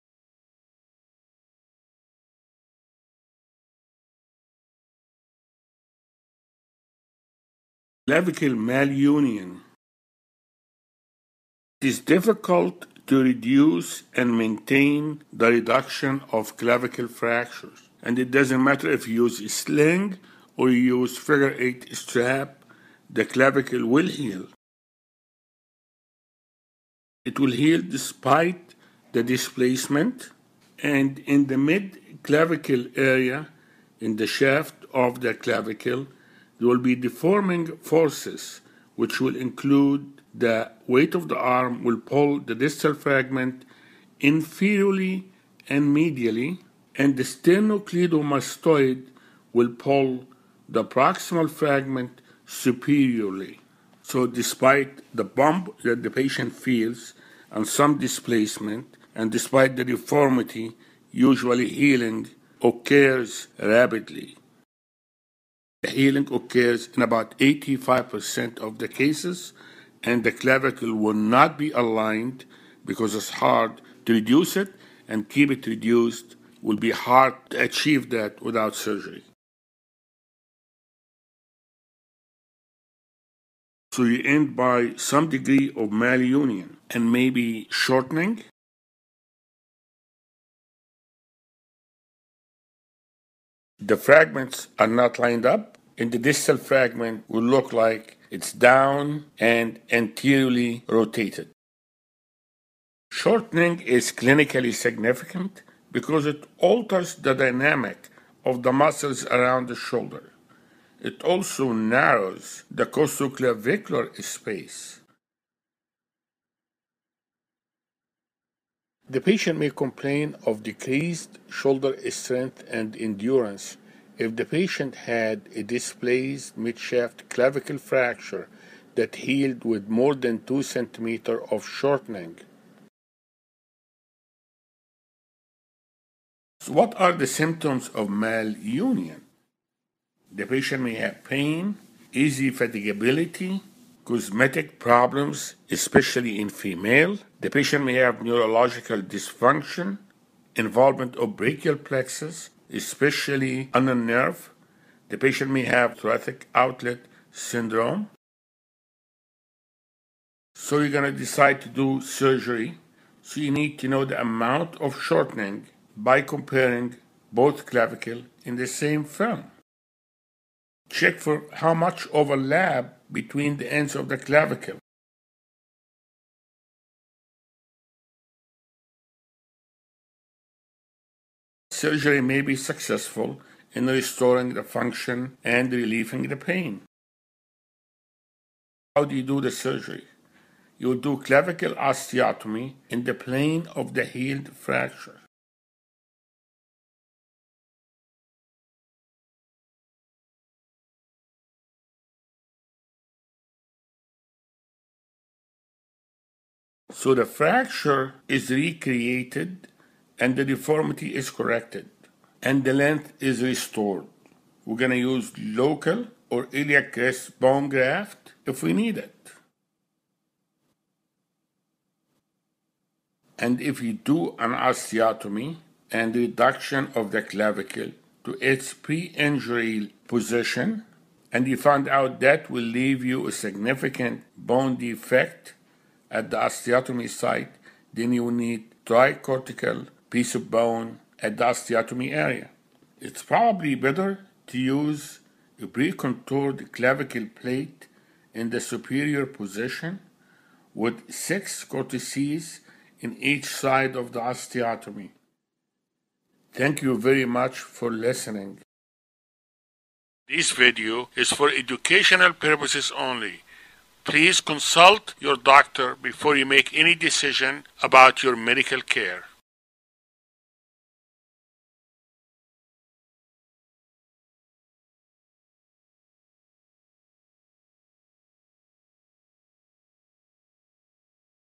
Malunion Of The Clavicle - Everything You Need To Know - Dr. Nabil Ebraheim
Dr. Ebraheim’s educational animated video describes malunion of the clavicle, explains the etiology and treatment of this condition and tells you all you need to know about it.
Regardless of a sling or figure 8 strap is used, the clavicle will heal.
The clavicle will heal despite the fracture displacement.
There will be deforming forces in the midshaft area of the clavicle.
Despite the presence of a bump, fracture displacement and deformity, healing of the fracture still occurs rapidly.
Healing occurs in about 85% of the cases; however the clavicle will not look aligned due to difficulty in reduction of the fracture.
It is hard to achieve reduction of the fracture without surgery.
Without surgical reduction, the fracture may end with some degree of malunion and possible of the clavicle.
The fragments will not line up with the distal fragment appearing to be downward and anteriorly rotated.
Shortening is clinically significant because it alter the dynamics of the muscles around the shoulder.
It also narrows the costoclavicular space.
The patient may complain of decreased shoulder strength and endurance if the patient had a displaced midshaft clavicle fracture that healed with more than healed with more than 2 cm of shortening.
What are the symptoms of clavicle malunion?
- Pain
- Easy fatigability
- Cosmetic problems (especially in females)
- Neurological dysfunction (possible involvement of the brachial plexus- especially the ulnar nerve)
- Patient may have thoracic outlet syndrome.
X-ray:
- Get x-ray of both shoulders: bilateral panoramic views to measure the shortening.
- Check the amount of overlap at the clavicle.
Treatment:
- Surgical treatment of the malunion may be successful in restoring the function and relieving the pain.
- Clavicle osteotomy is done in the plane of the healed fracture.
- The fracture is recreated with correction of the deformity and the length of the clavicle is restored.
- If an osteotomy is done with reduction of the clavicle to its preinjury position and this leaves a significant bone defect, then you will need a tricortical piece of bone at the osteotomy area.